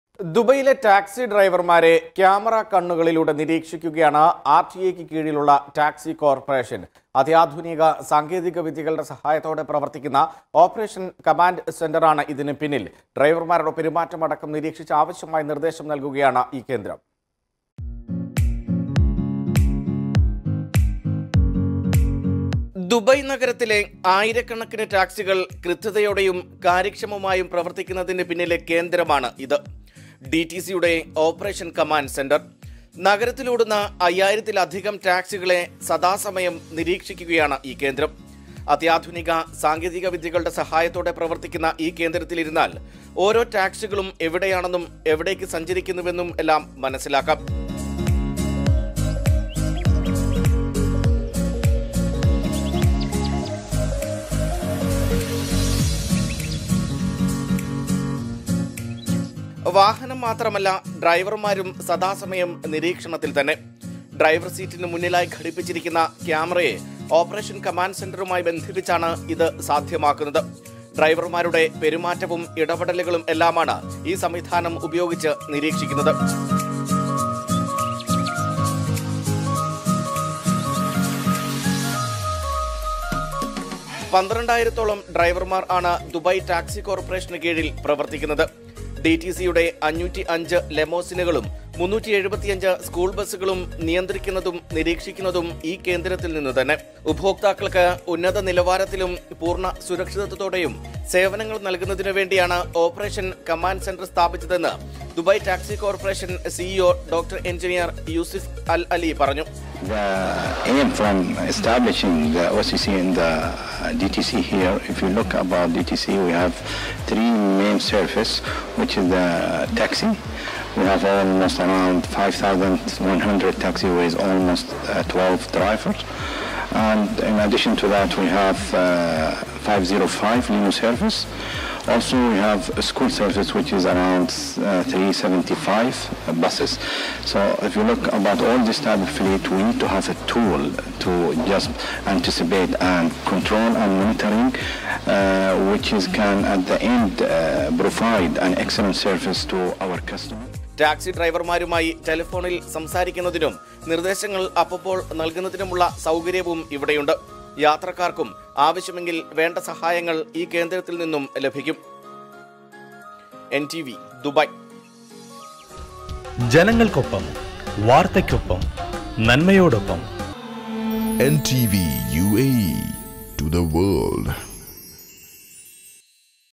ടാക്‌സി ഡ്രൈവര്‍മാരെ ക്യാമറാക്കണ്ണുകളിലൂടെ നിരീക്ഷിക്കുകയാണ് ഓപ്പറേഷന്‍ കമാന്‍ഡ് സെന്റര്‍
ദുബൈയിലെ ടാക്‌സി ഡ്രൈവര്‍മാരെ ക്യാമറാക്കണ്ണുകളിലൂടെ നിരീക്ഷിക്കുകയാണ് ആര്‍ടിഎയ്ക്ക്
കീഴിലുള്ള ടാക്‌സി കോര്‍പ്പറേഷന്‍.
അത്യാധുനിക സാങ്കേതിക വിദ്യകളുടെ സഹായത്തോടെ പ്രവര്‍ത്തിക്കുന്ന ഓപ്പറേഷന്‍ കമാന്‍ഡ് സെന്റര്‍ ആണ് ഇതിന് പിന്നില്‍.
ഡ്രൈവര്‍മാരുടെ പെരുമാറ്റം അടക്കം നിരീക്ഷിച്ച് ആവശ്യമായ നിര്‍ദ്ദേശങ്ങള്‍ നല്‍കുകയാണ് ഈ കേന്ദ്രം